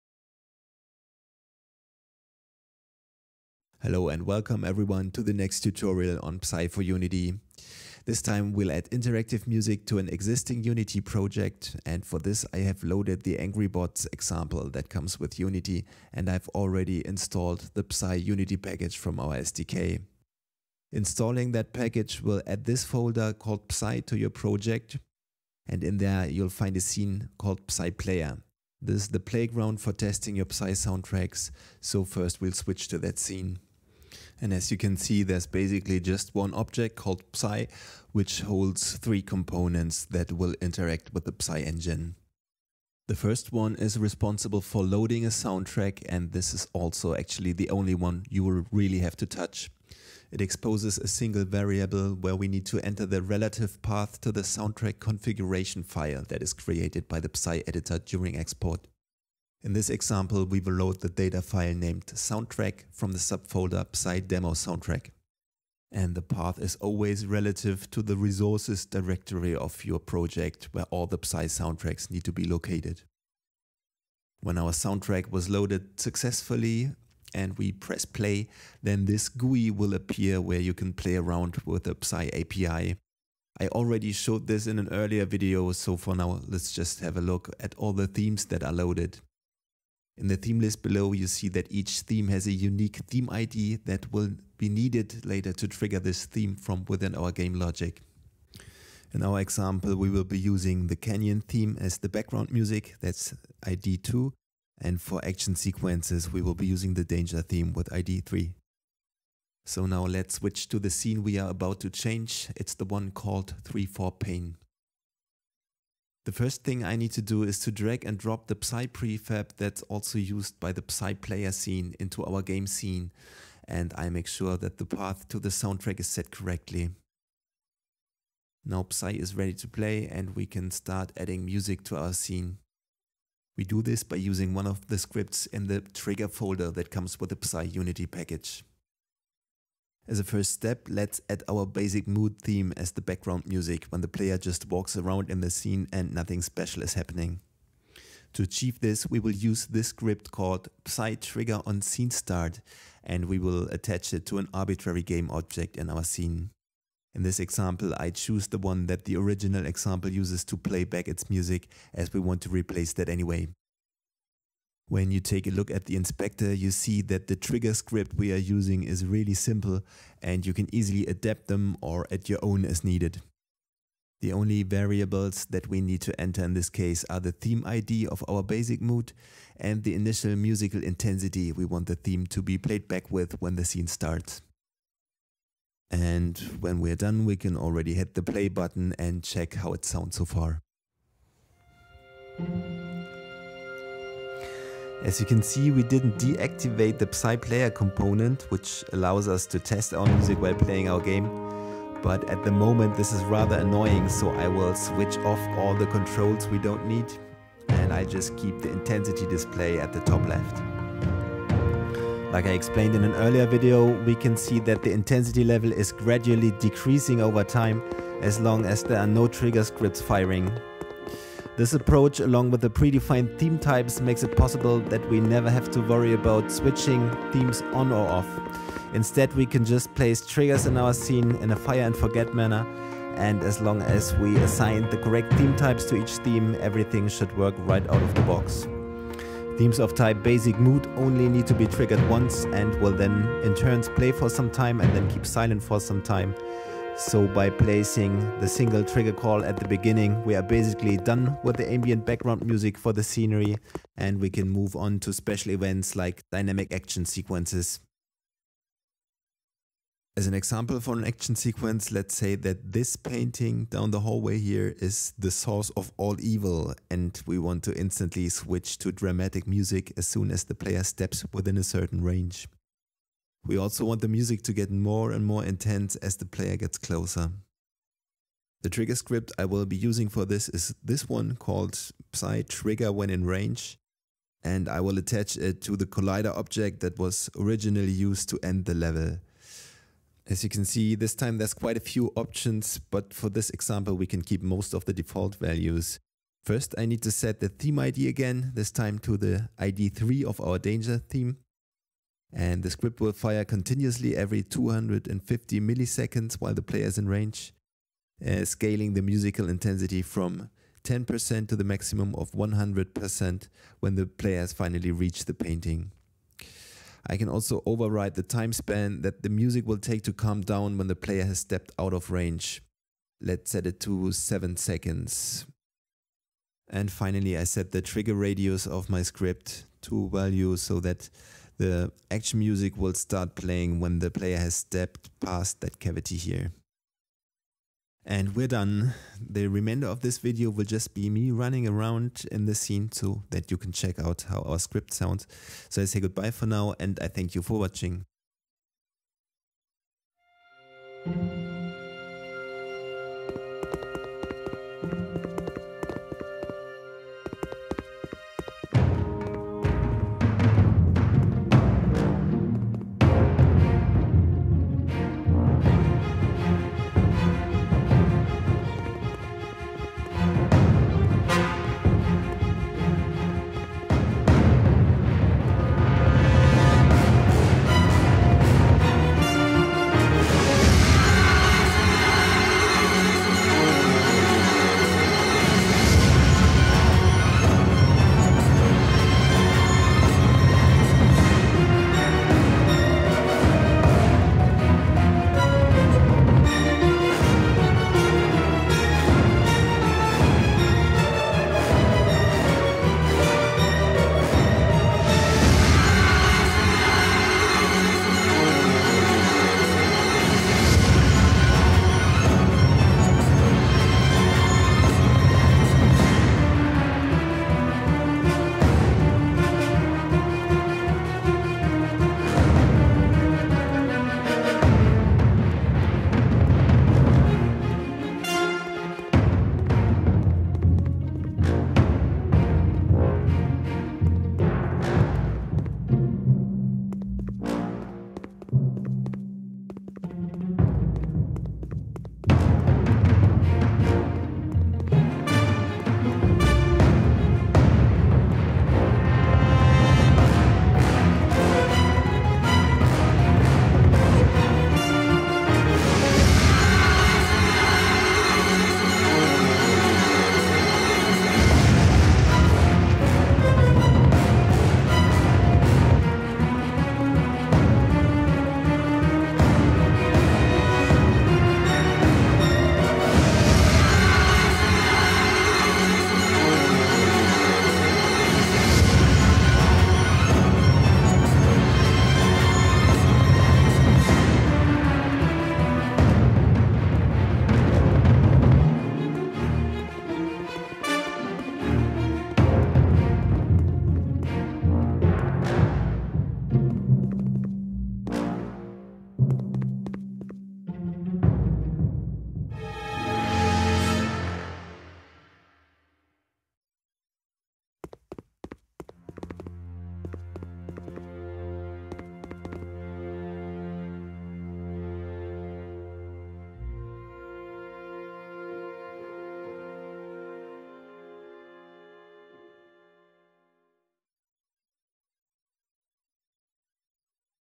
psai® Tutorial: How to integrate your soundtrack into your Unity game
psai® is the first dedicated Interactive Music Engine for videogames
available on Unity and PC.